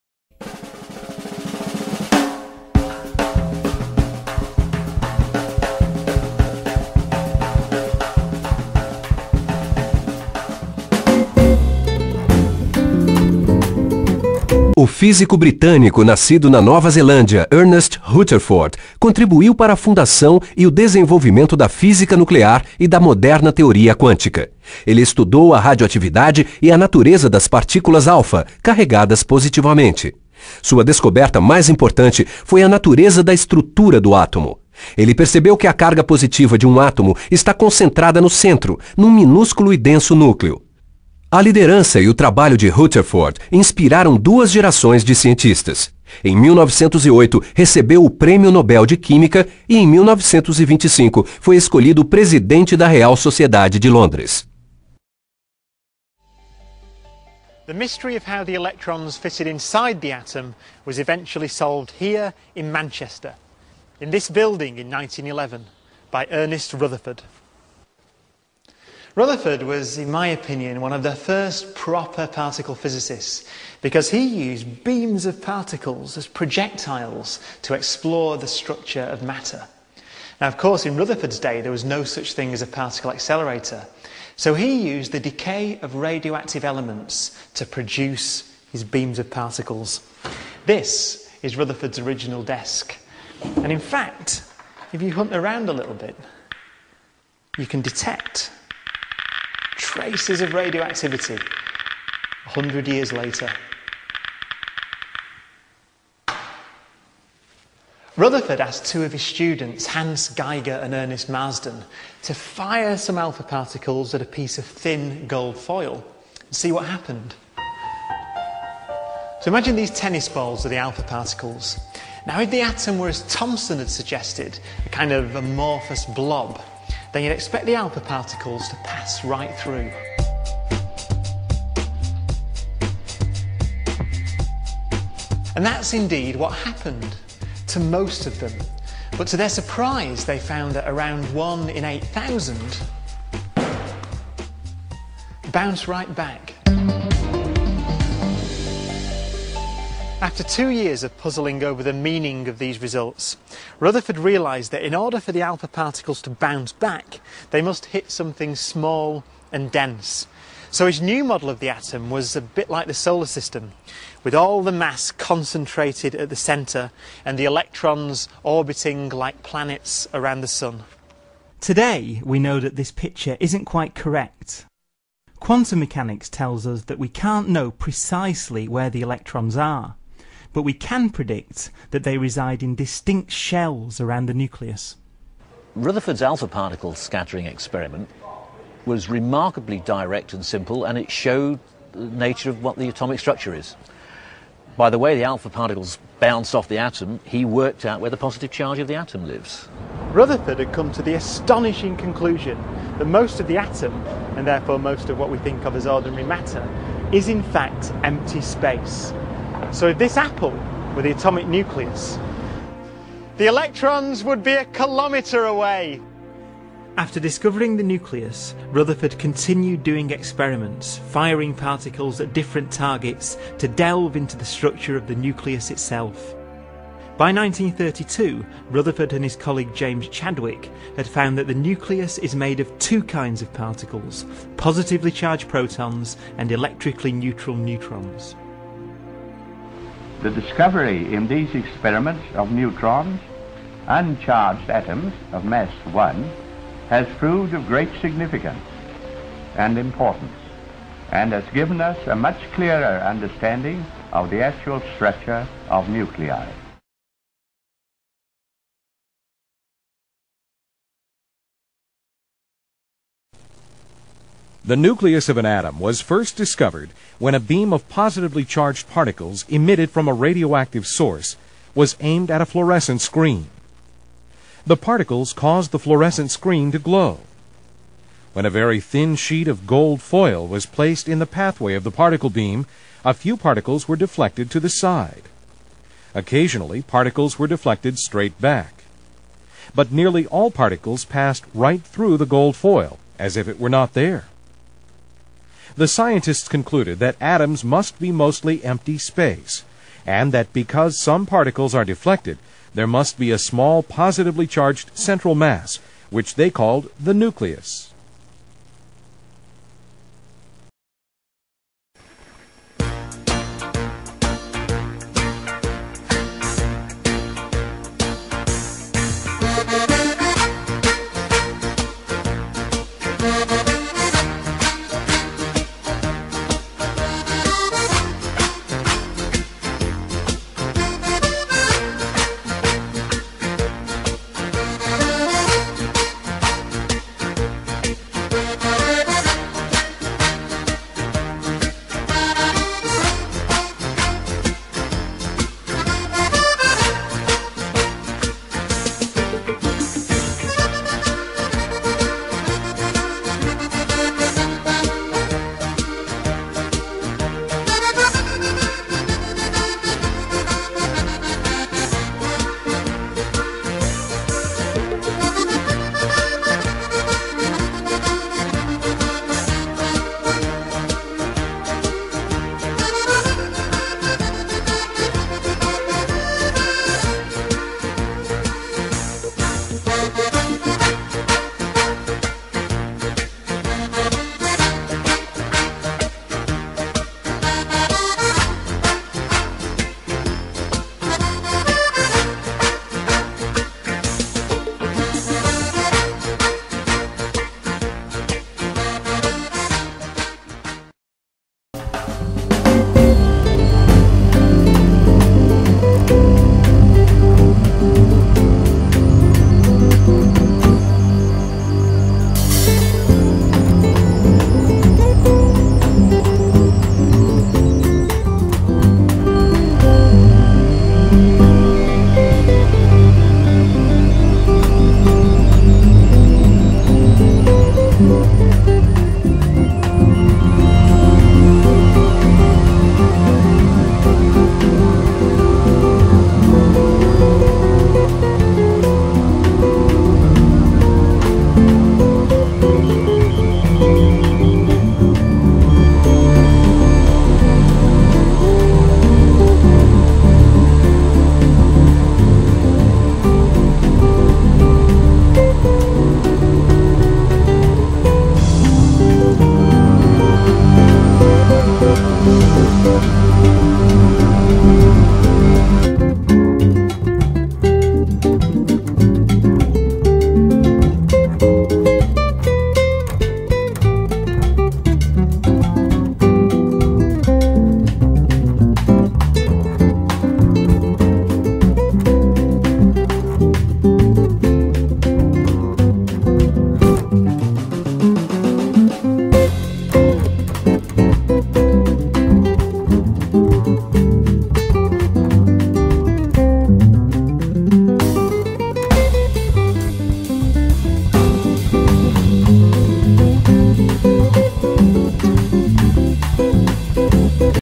Modelo atômico-Rutherford
Sequência de vídeos: Modelo de Rutherford. Faz parte do projeto MedQuímica no Youtube, dos professores Alex, Duda e Tasso.